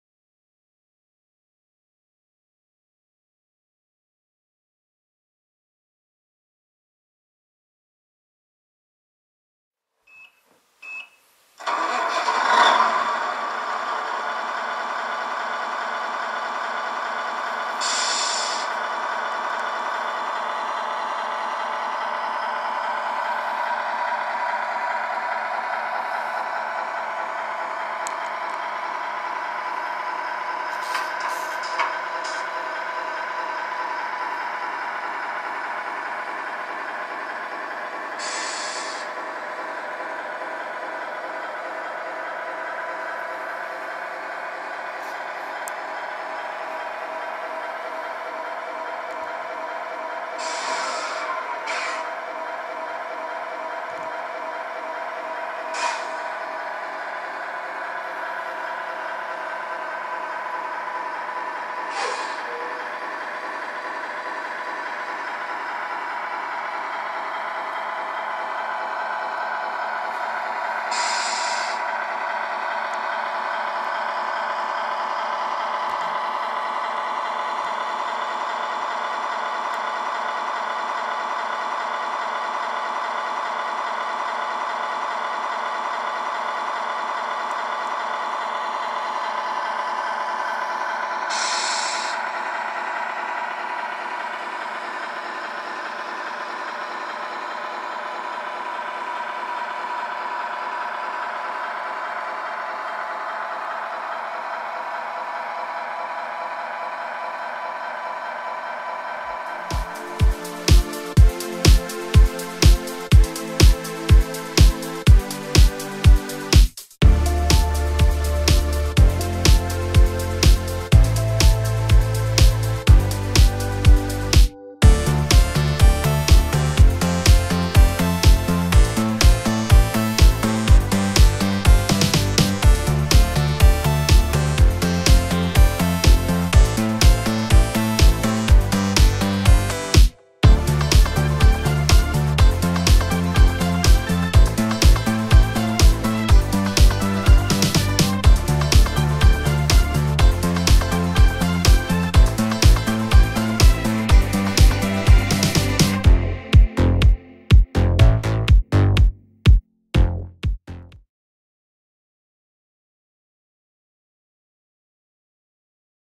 【Knight #6】 ( 動作確認 ) ナイトハウラー[ Tamiya Knight hauler ]( ラジコン トレーラー )
バイザーの製作
ボディーの再塗装を行いました。

バイザーはアルミで製作、
バイザー、ボディー共に

パテ埋め　→２週間後、６００番耐水ヤスリ　→１０００番耐水ヤスリ

→１５００番耐水ヤスリ　→サーフェイサー砂吹き

→サーフェイサー（計２回）

→１週間後１０００番耐水ヤスリ

→１５００番耐水ヤスリ　→２０００番耐水ヤスリ（下地完了）

→黒砂吹き塗装　→１５分後、黒塗装

→１週間後、黒塗装→１週間後、黒塗装（計４回）

→１週間後、クリア砂吹き塗装　→１５分後、クリア塗装

→１週間後、クリア塗装　→１週間後、クリア塗装（計４回）（塗装完了）

→１ヶ月後、２０００番耐水ヤスリ　→６０００番耐水ヤスリ

→８０００番耐水ヤスリ　→コンパウンド細め　→コンパウンド仕上げ目

→コンパウンド水性仕上げ目

→コーティングポリマー（全体完成）

このように、大変時間がかかります（特に暗い色）、
方法は人それぞれですが、参考までに。